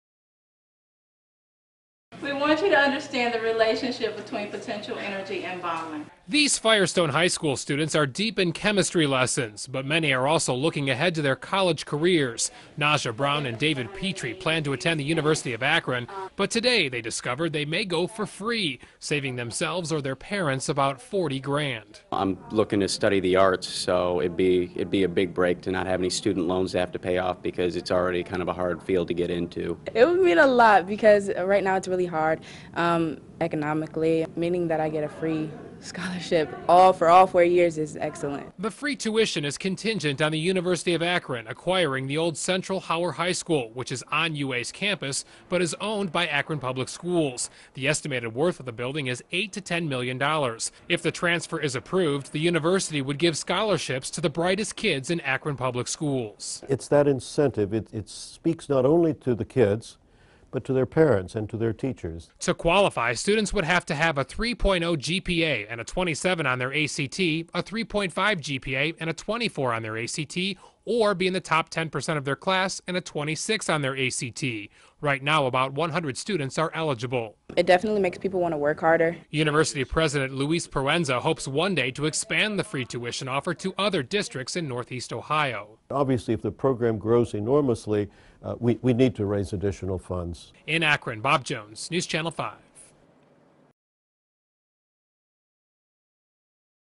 Free Tuition
University of Akron and Akron Public Schools working on a deal for scholarships in a swap for Central Hower High School building.